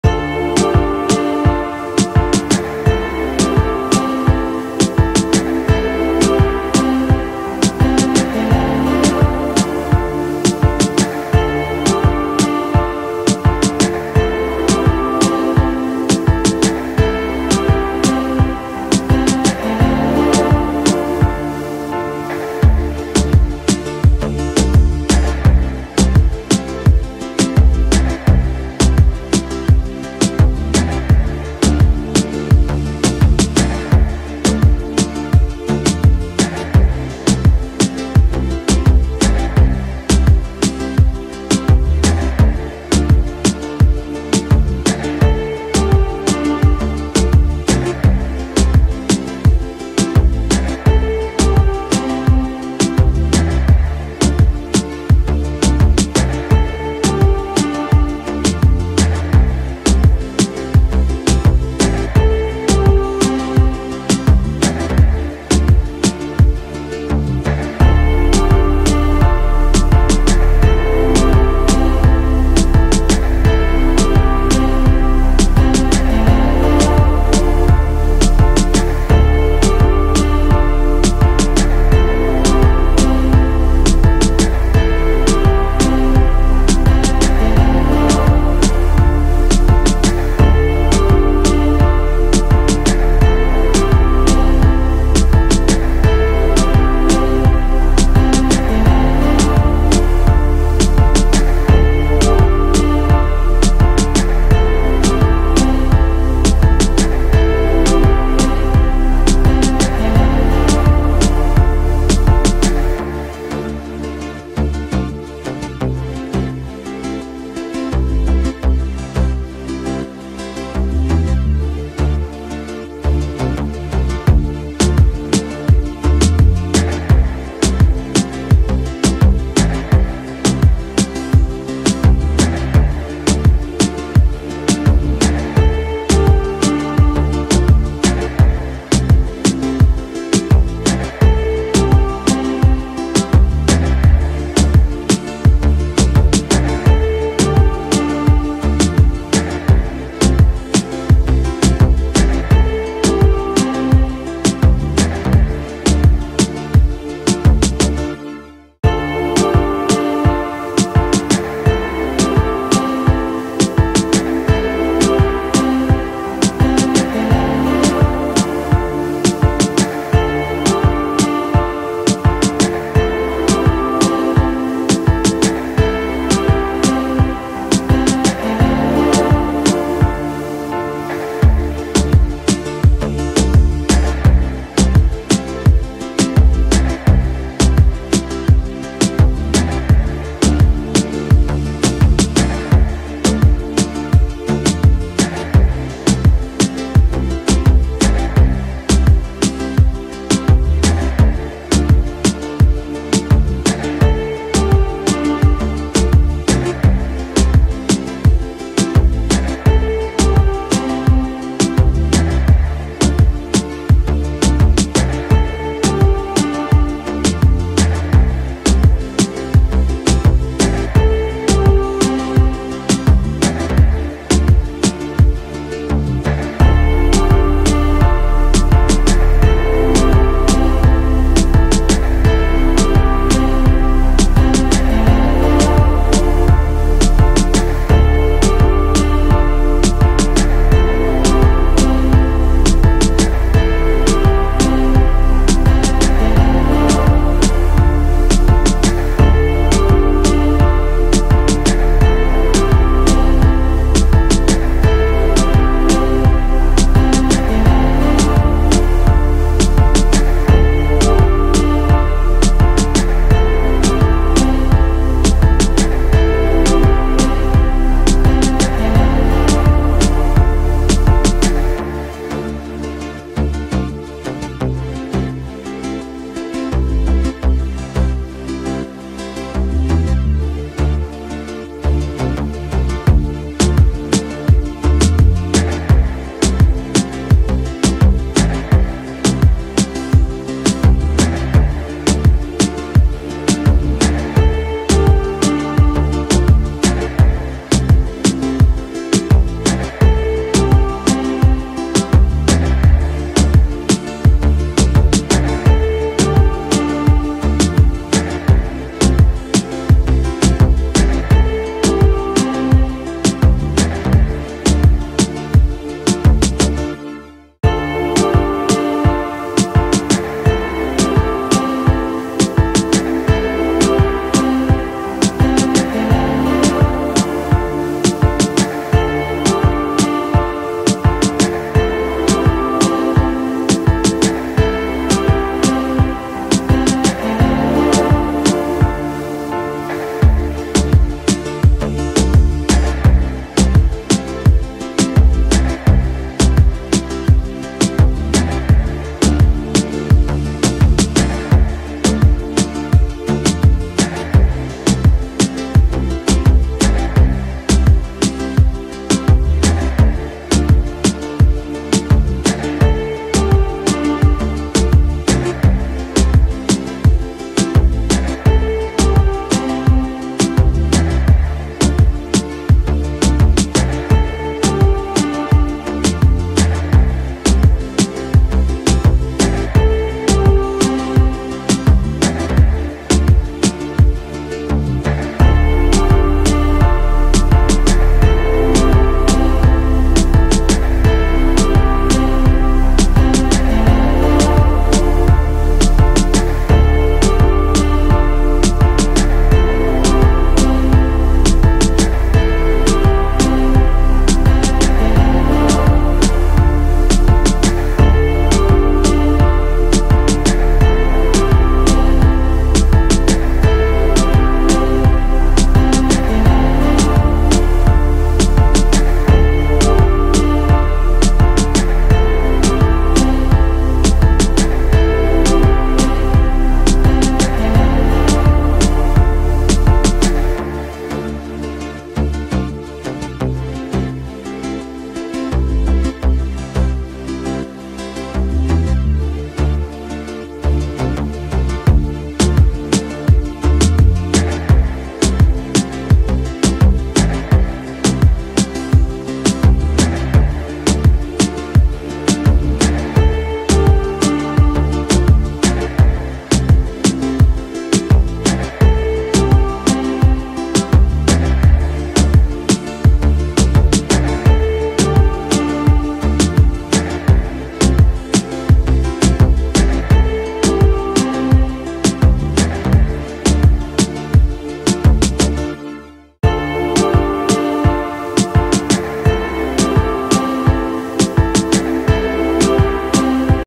Mila Marwah VS Kids Oliver Transformation 2024 ★ From Baby To Now
* Who is  Mila Marwah ?
Social media star who was famous from birth as the daughter on the Anasala Family YouTube channel. The vlogs uploaded by her parents are watched by over 14 million subscribers. Her parents often focus videos on different milestones and events in her life.

* Who is Kids Oliver?
Ukrainian social media star who has gained fame for his Kids Roma Show YouTube channel. His toy reviews, unboxings, kid-friendly vlogs and hauls have earned him more than 11 billion cumulative views.